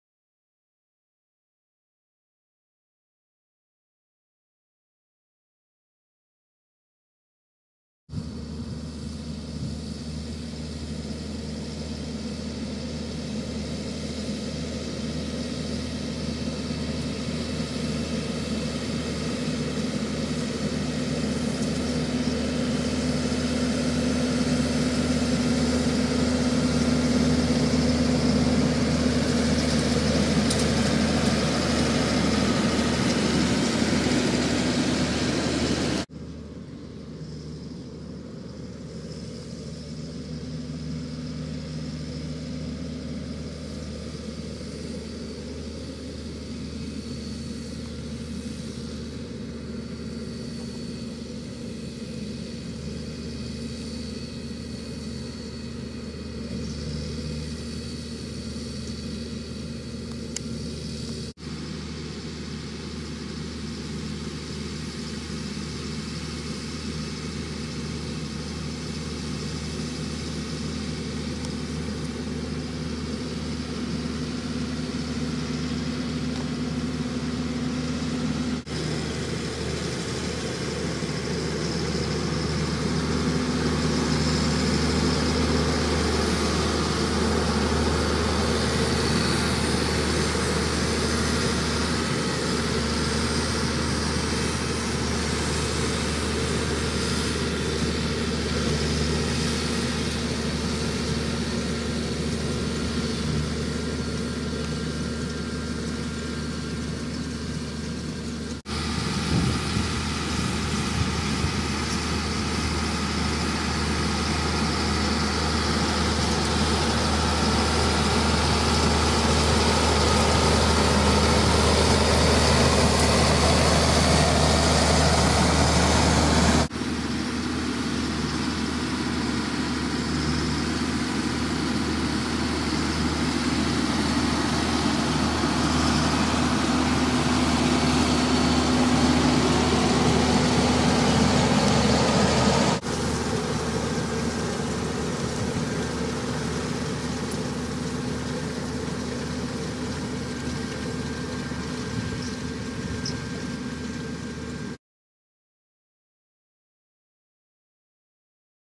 Cornish Farmers & Contractors Silage 2012 Fendt 820
Part5 of Wills Bros Ltd Silage. In another field caught this little beauty working away, Fendt 820 & Claas Liner 2900 rake...